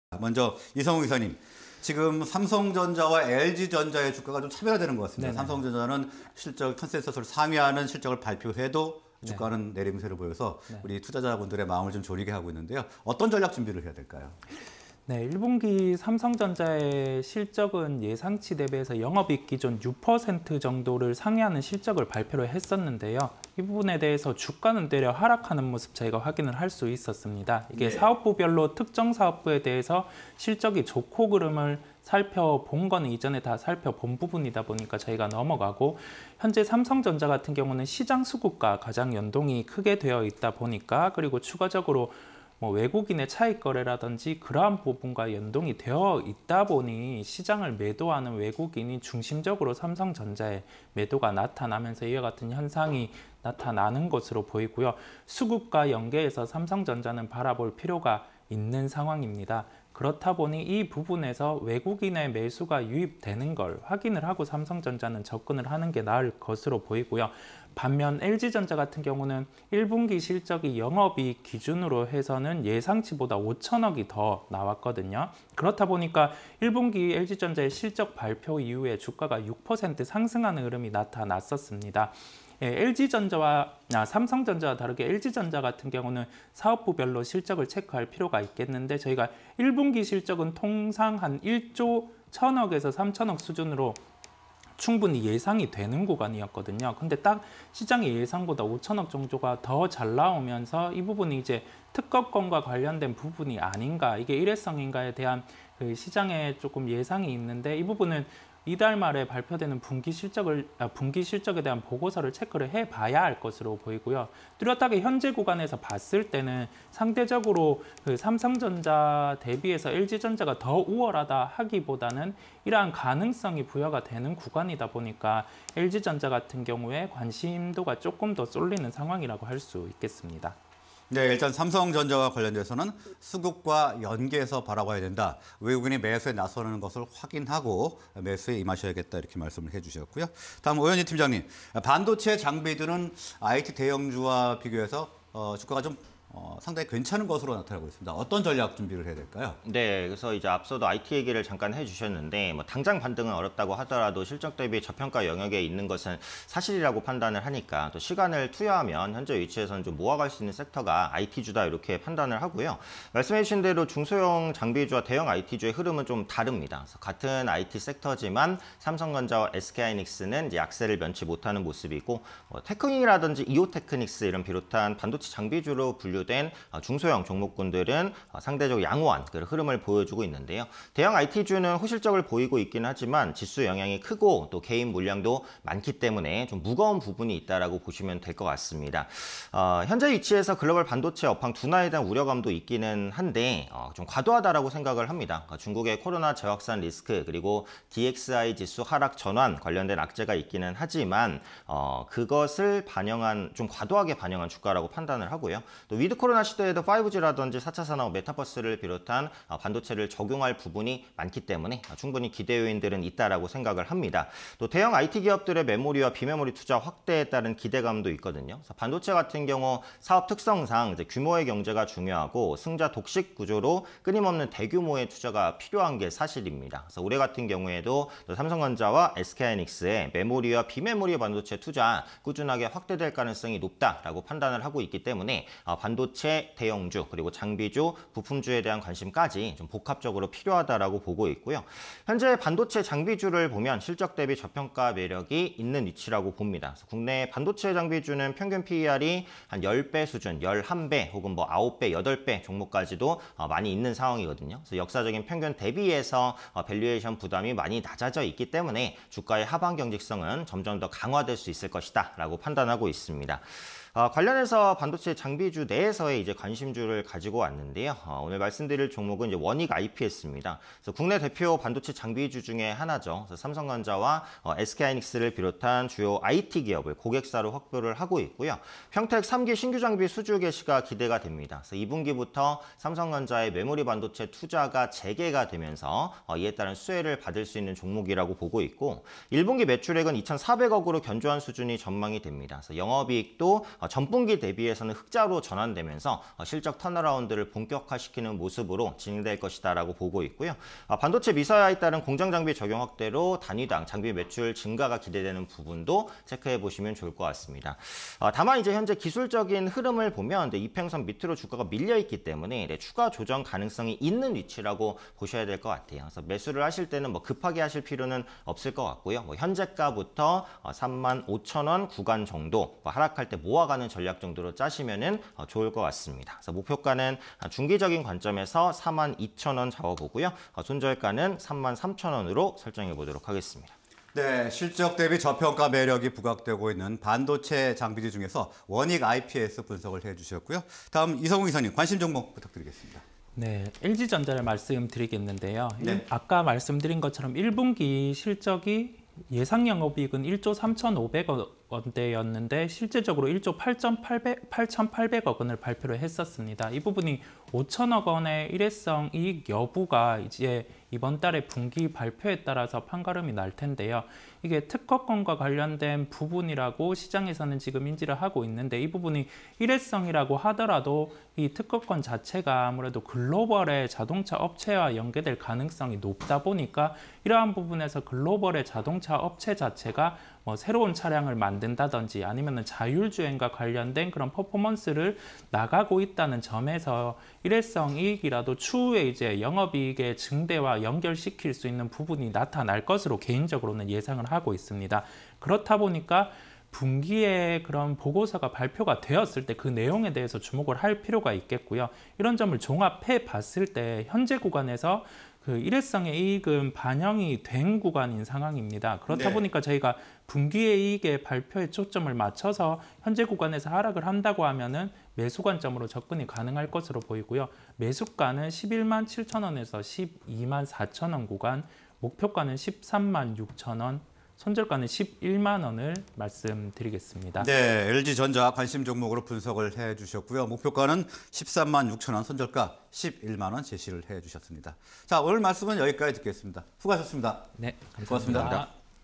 LG전자(066570)... 1분기 매출·영업이익 사상 최대 / 원익IPS(240810)... 삼성전자향 신규 장비 수주 기대_개장 관심주 (20220412)
Q. 삼성전자-LG전자 주가 차별화, 시사점은?
- 삼성전자, 1분기 영업이익 예상치 대비 6% 상회
- 삼성전자, 수급 측면 매도 지속
- LG전자, 1분기 실적 발표 이후 6% 상승
- LG전자, 예상치 상회한 실적 사업부 확인

Q. 반도체 장비주, 주가 '견조'… 기대요인은?
- 중소형 장비주, 대형 IT주와 흐름 차별화
- 글로벌 반도체 수요 둔화 우려 과도
- 대형 IT기업, 메모리∙비메모리 투자 확대 수혜
- 반도체 장비주, 실적 대비 저평가 매력 부각

LG전자(066570)
- 1분기 매출·영업이익 사상 최대
- 일회성 이익 여부에 주목

원익IPS(240810)
- 삼성전자향 신규 장비 수주 기대
- 1분기 영업익, 전분기대비 흑자전환 전망
- 반도체 미세화 따른 공장장비 적용 확대